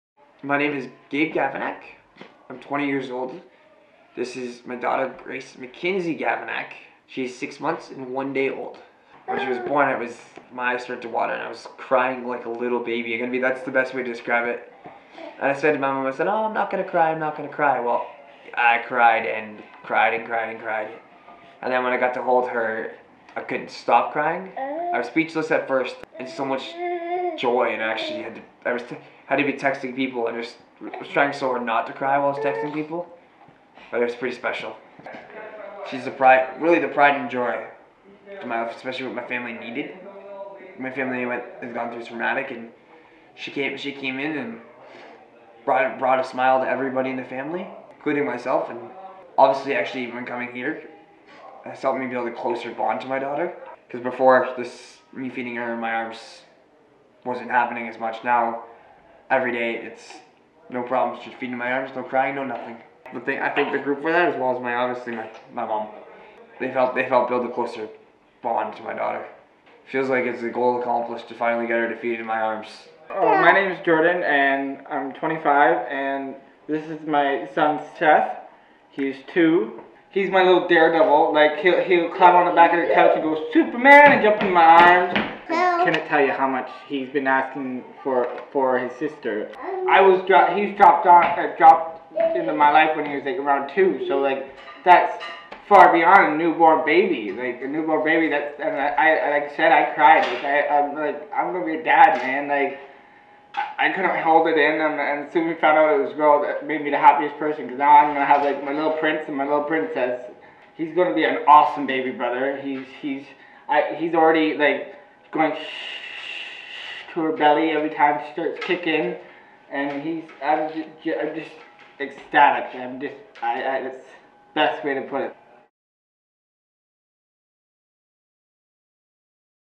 Young Dads
Young fathers meet to talk about parenting, their kids and guy stuff.